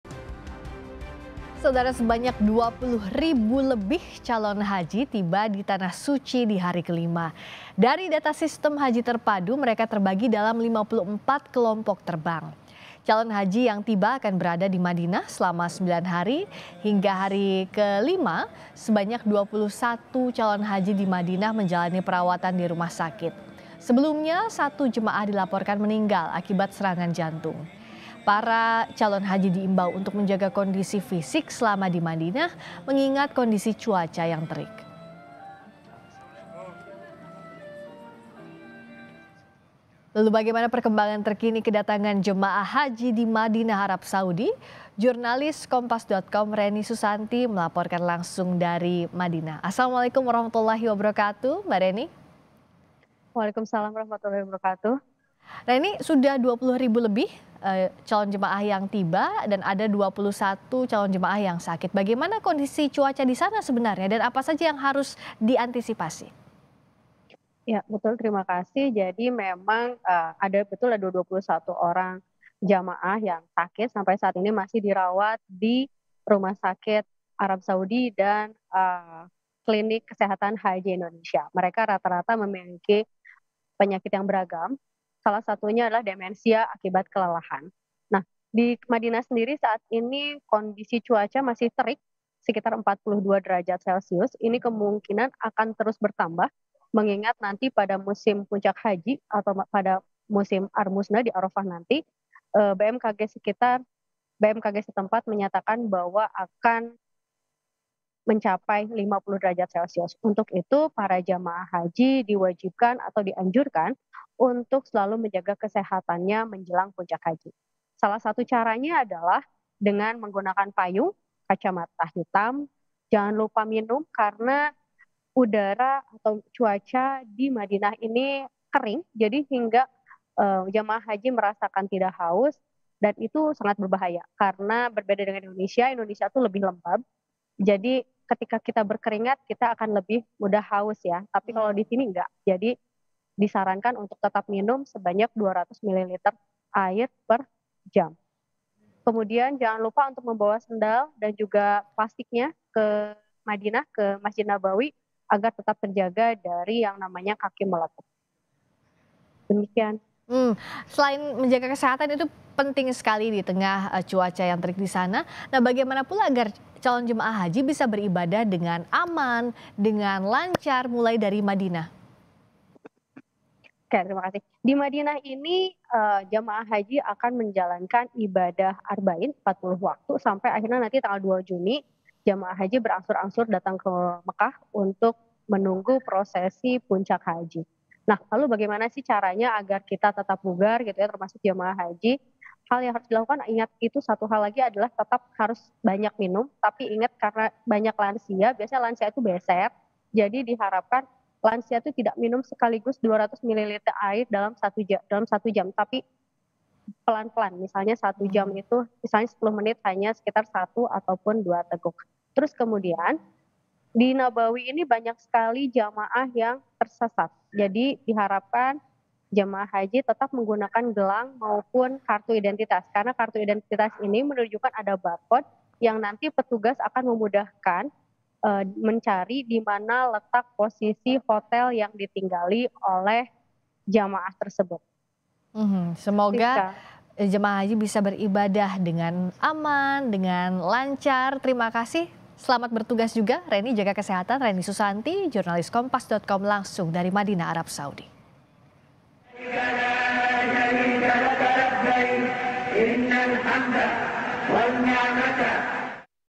21 Calon Haji Indonesia Dirawat di Rumah Sakit Setibanya di Madinah
MADINAH, KOMPAS.TV - Sebanyak 20.000 lebih calon haji tiba di tanah suci di hari ke 5.

Dari data sistem Haji Terpadu, mereka terbagi dalam 54 kelompok terbang.

Calon haji yang tiba akan berada di Madinah selama 9 hari.

Hingga hari ke-5, sebanyak 21 calon haji di Madinah menjalani perawatan di rumah sakit.

Sebelumnya 1 jemaah dilaporkan meninggal akibat serangan jantung.

Para calon haji diimbau untuk menjaga kondisi fisik selama di Madinah mengingat kondisi cuaca yang terik.

Baca Juga Kisah Tini, Perempuan yang Mengabdikan Dirinya Lestarikan Owa Jawa Sealam 8 Tahun! di